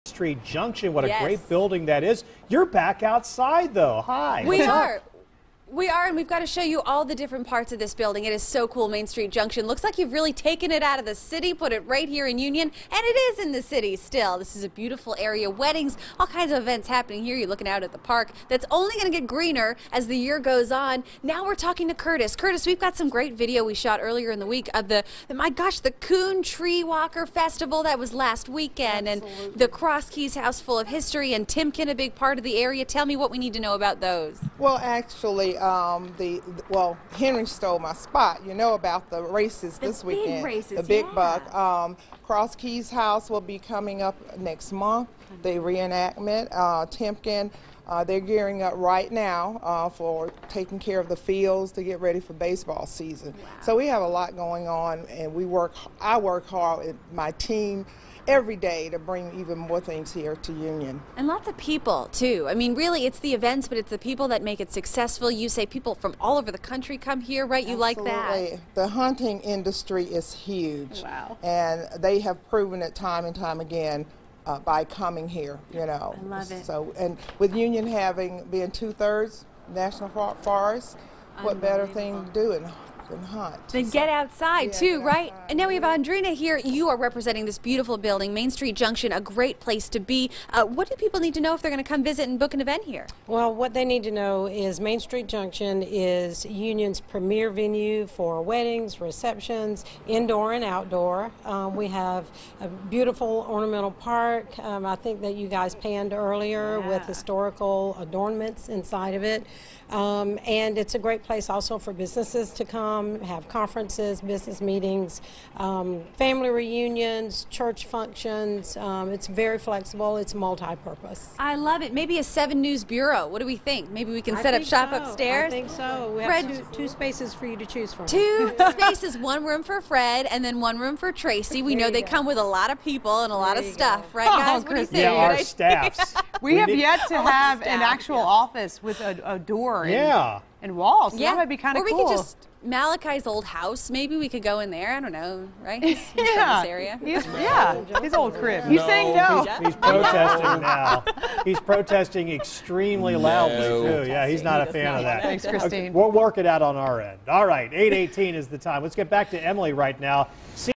Hometown Spotlight Union
Friday morning on 7 News Hometown Spotlight our crew paid a visit to Union, South Carolina.